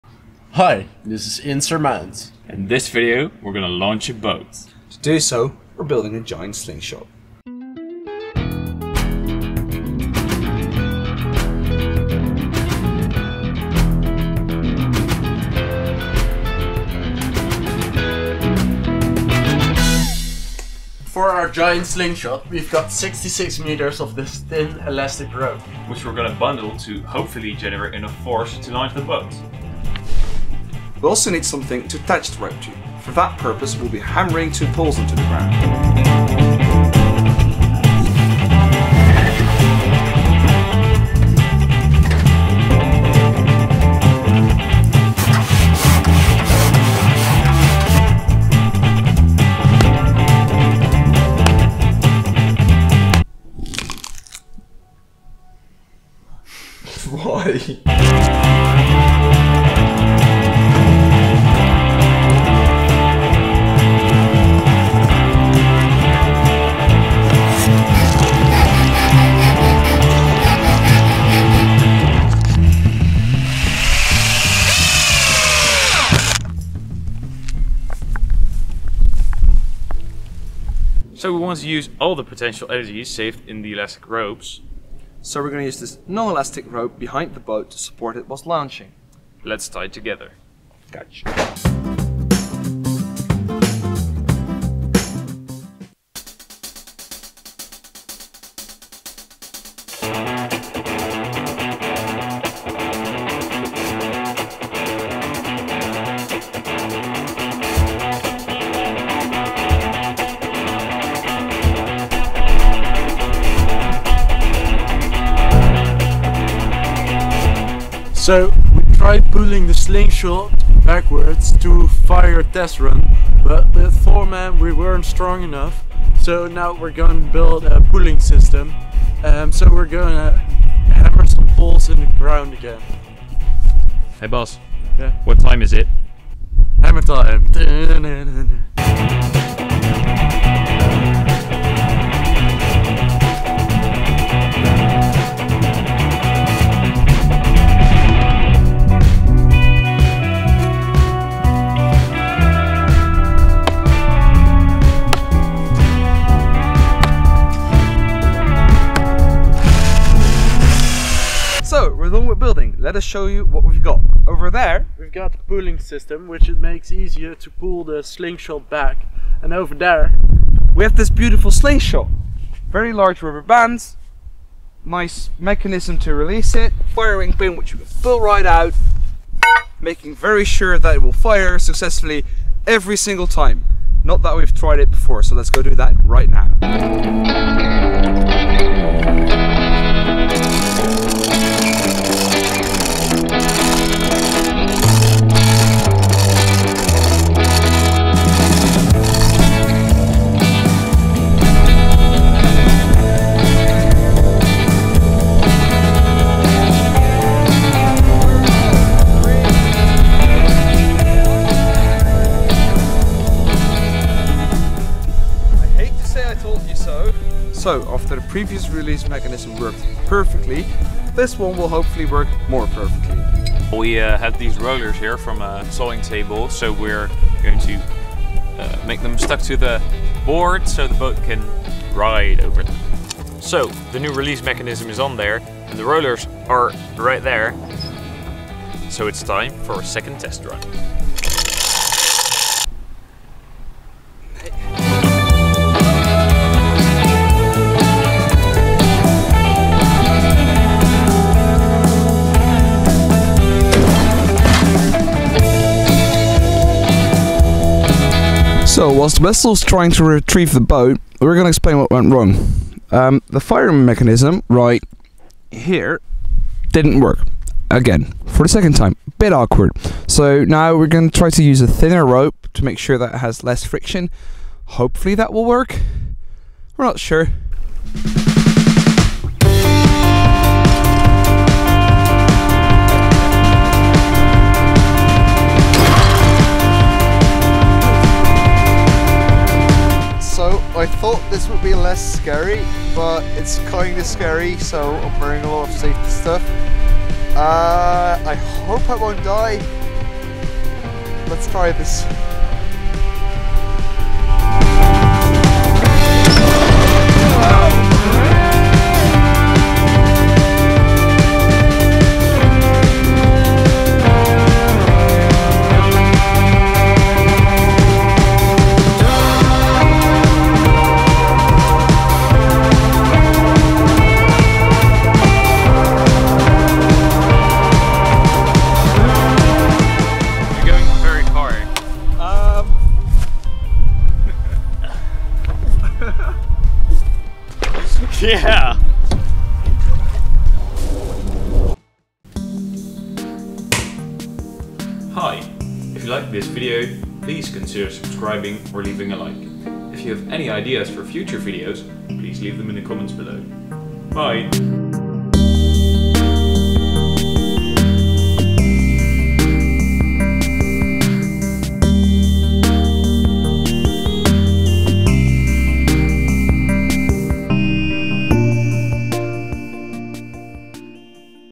REAL Giant slingshot | launching ourselves in a BOAT!
Yup, in this video we launch ourselves with a giant slingshot.
Its not something to try at home, unless you really want to win that darwin award.